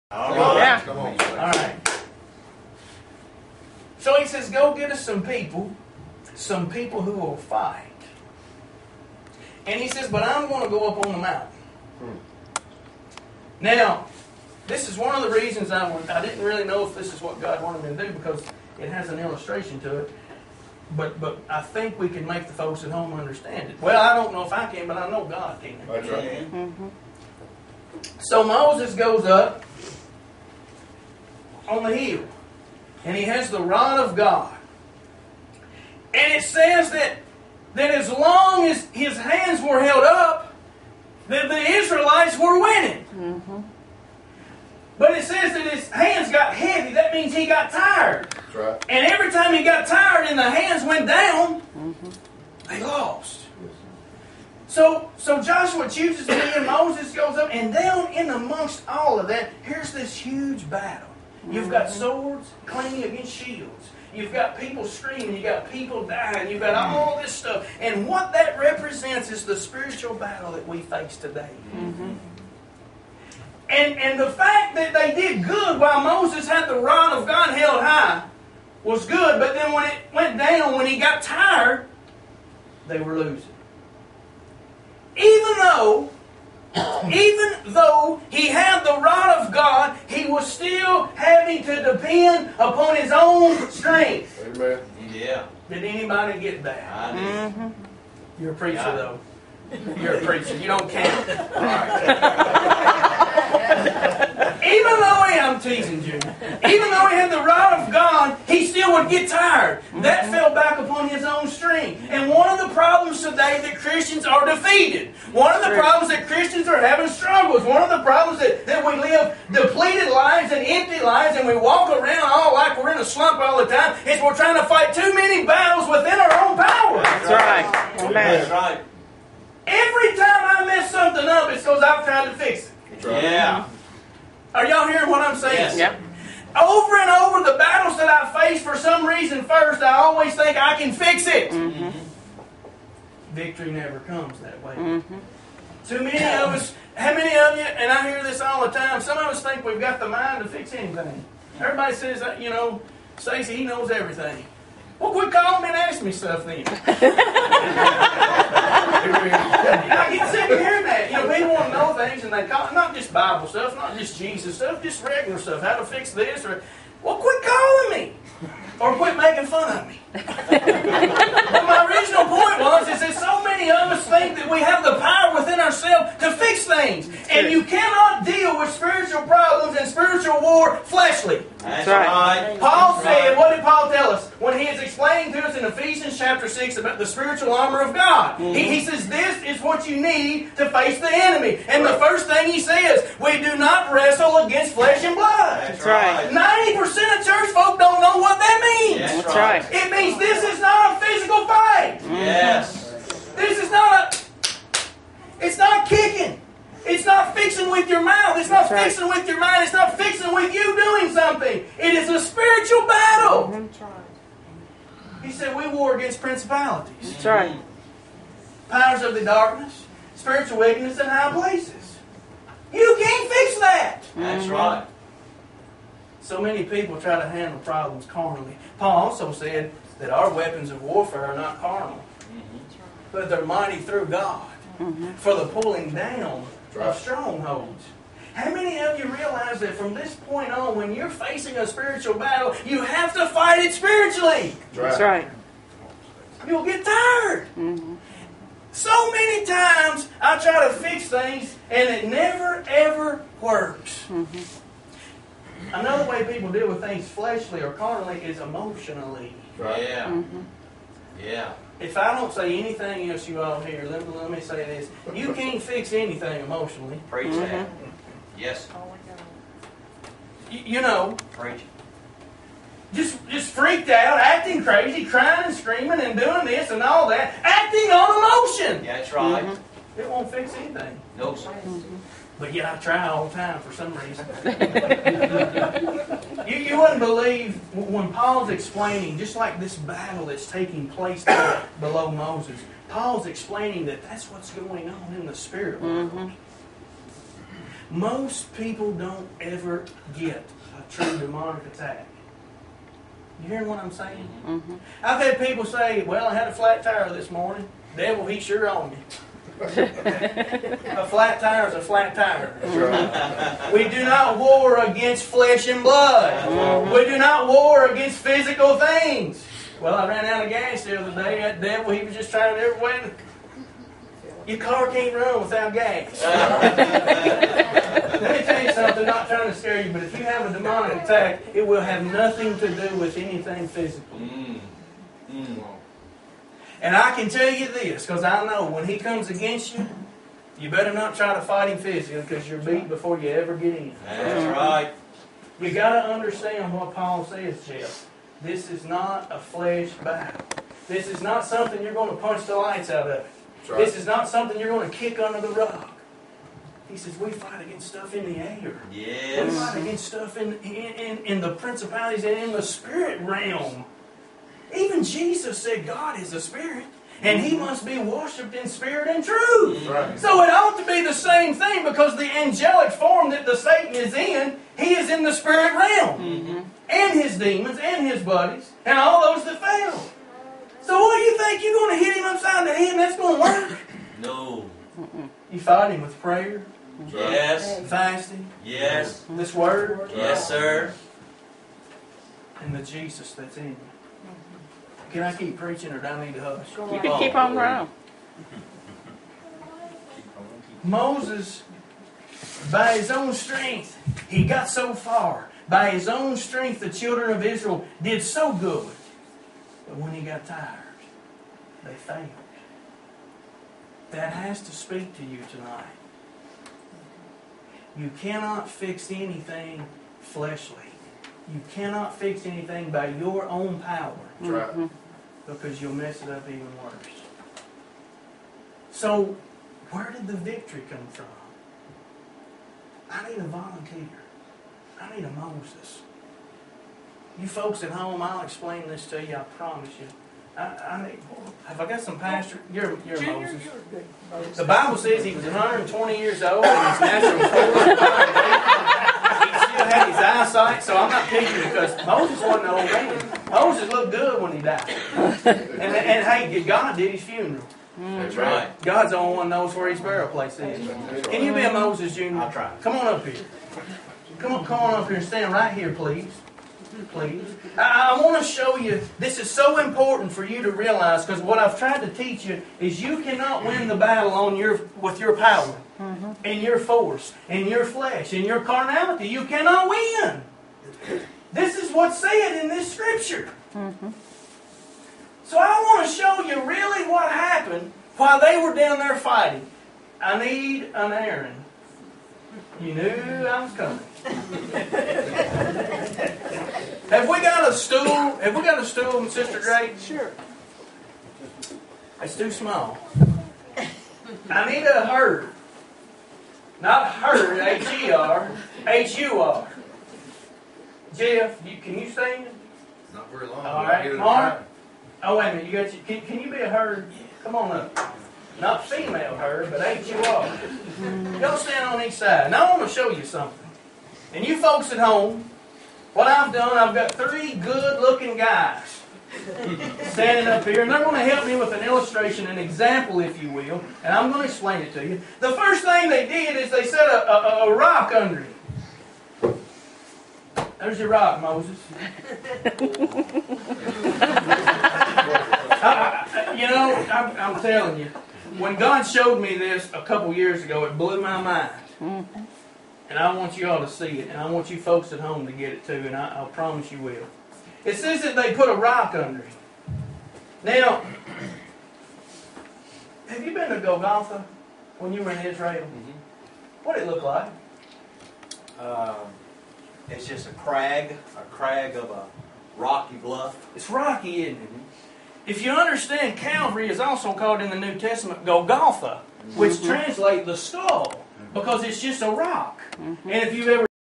Stacy Wilson Part 4.flv
Stacy Wilson Part 4, preaching at the WDFB Radio Revival, February 2010.  WDFB Christian Radio, 1170 AM, 88.1 FM, Danville, Ky.